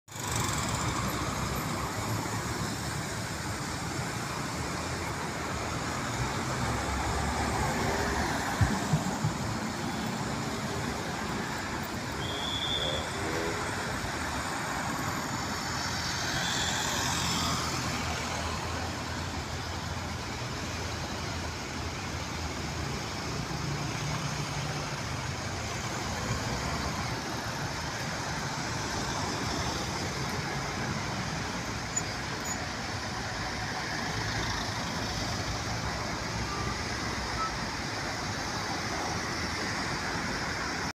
Karl Topia Square
brings you this short video in Tirana Downtown during a cool Thursday morning.